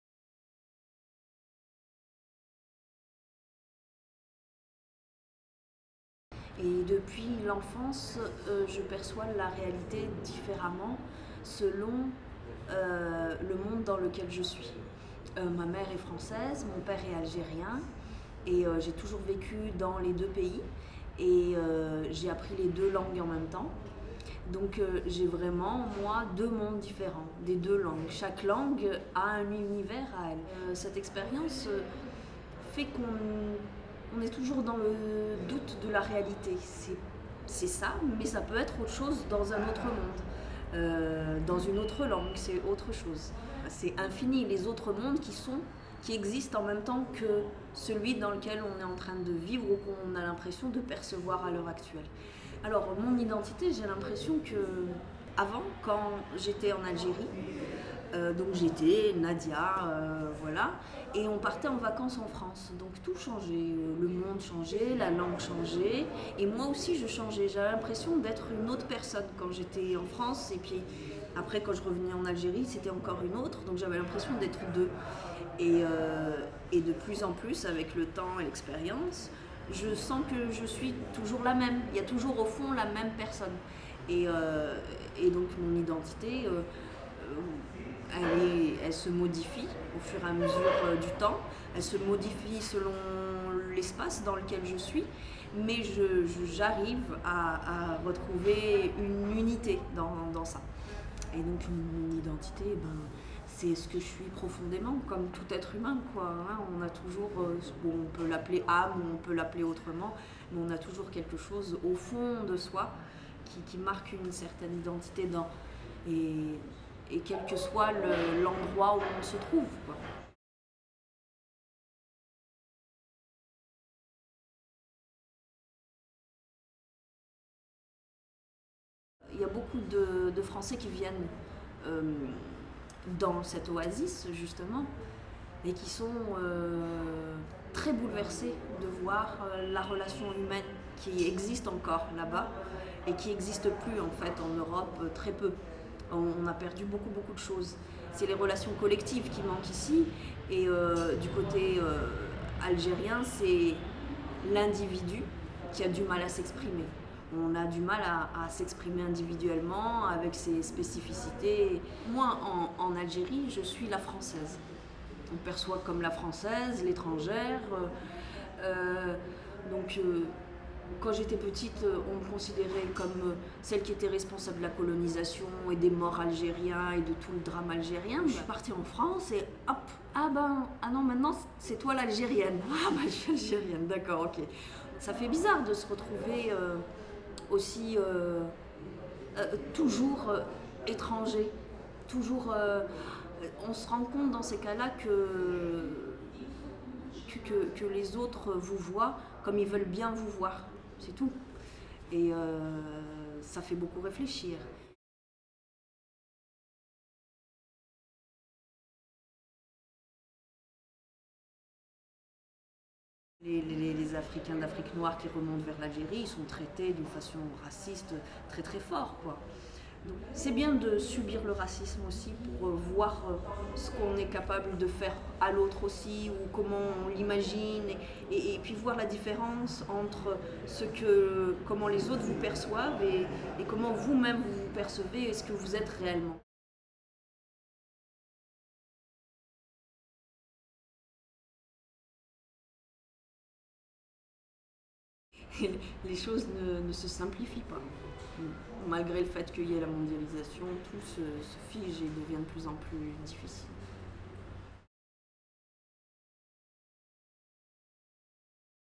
Nadia Chouieb
Interview in French - Italian subtitles

Regista - Francia/Algeria
Luoghi Comuni: la parola ai registi
XX Festival del Cinema Africano, d'Asia e America Latina
Milano, 2010
Produzione: Kenzi Productions in collaborazione con Lettera 27

Film director - France/Algeria
XX Festival of African, Asian and South American Cinema
Milan, Italy - 2010
Production: Kenzi Productions